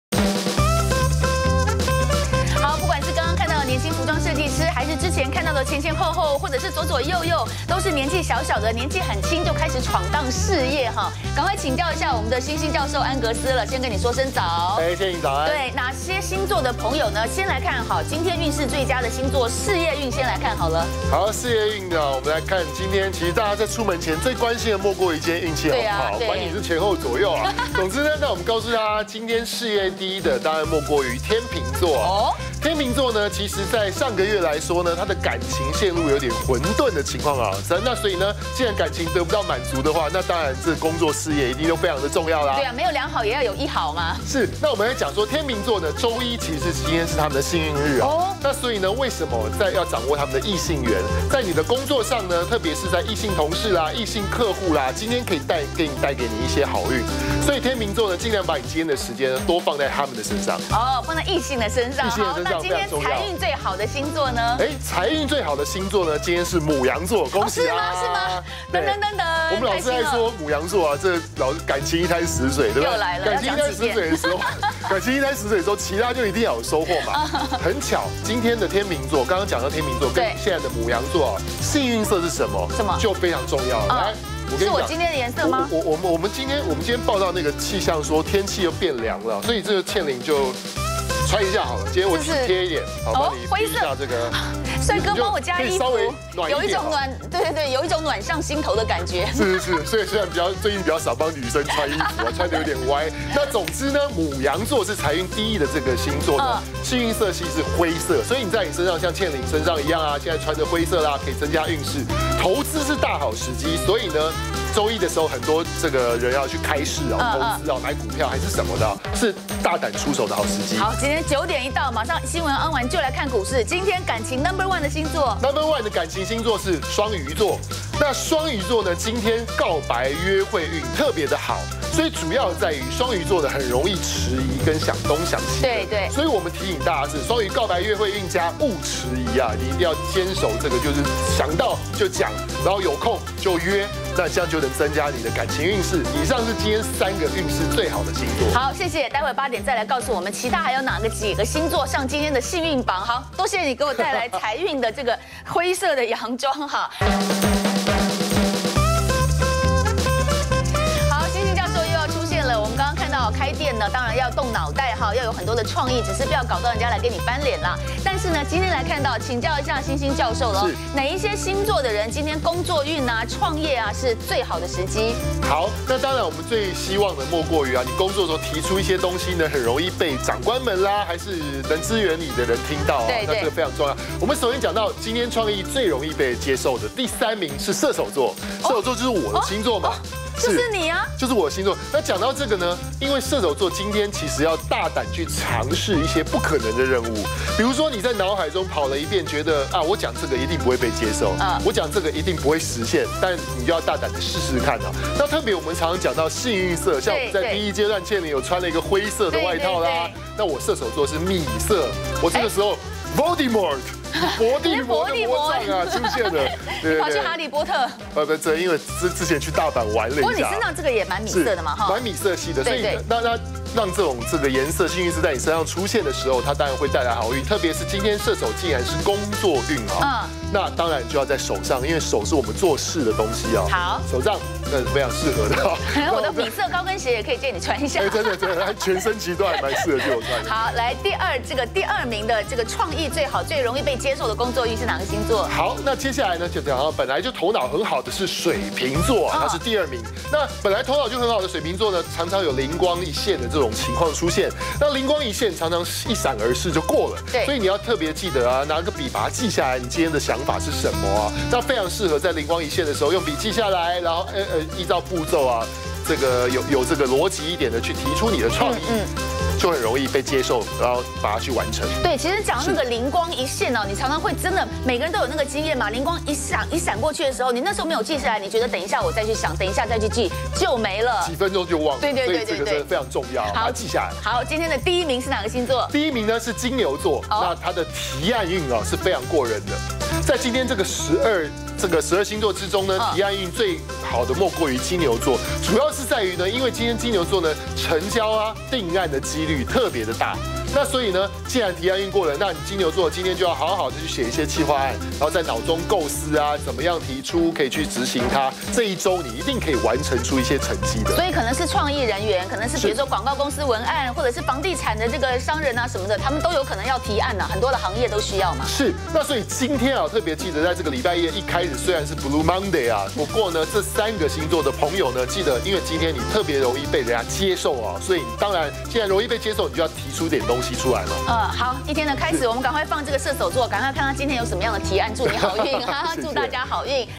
20151012安格斯星座 事業NO.1天秤週一幸運日 掌握異性緣
運勢最佳星座，事業NO.1天秤週一幸運日，掌握異性緣，財運NO.1牡羊投資大好時機，大膽出手，感情NO.1雙魚告白約會運佳，勿遲疑。創意佳NO.3射手大膽嘗試，不可能的任務，創意佳NO.2水瓶靈光乍現，適合提出創意，創意佳NO.1金牛提案運過人，成交機率越大。